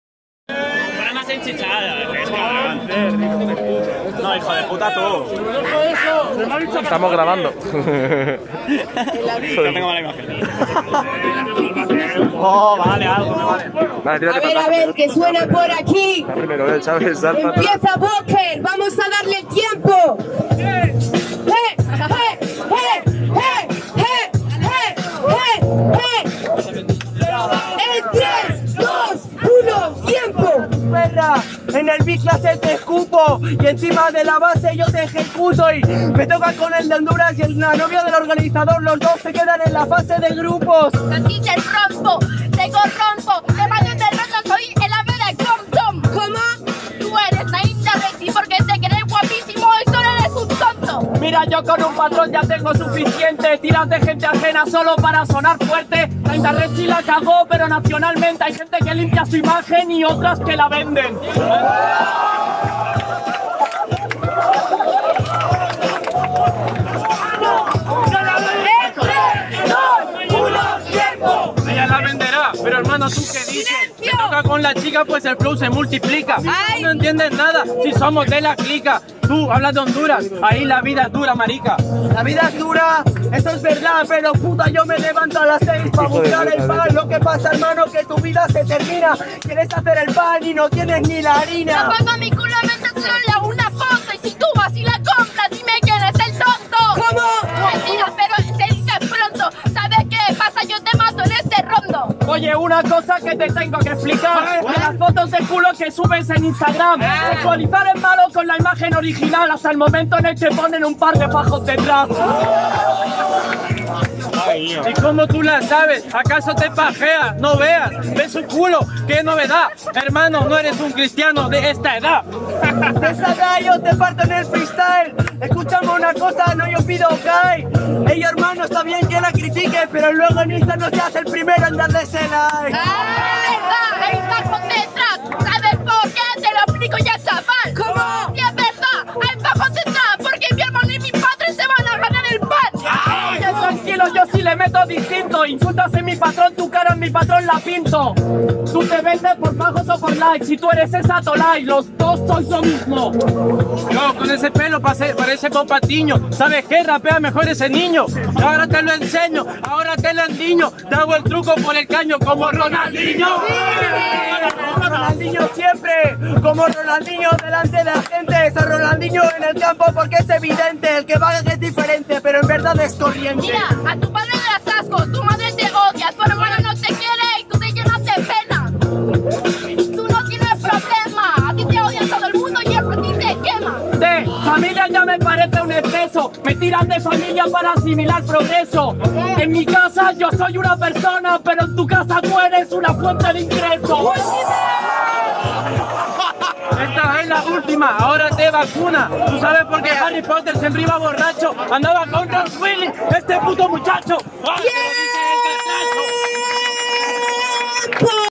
(😡BEEF😡) PIROMANA ANDINO vs ANTIC BOKER | Clasificatoria a GRAND BATTLE III
Evento clasificatorio a la nacional de GRAND BATTLE III organizada en BENIDORM por @TodoRap Benidorm
Instagram Speaker @marina_vinyals
Instagram MVP @mvpbattles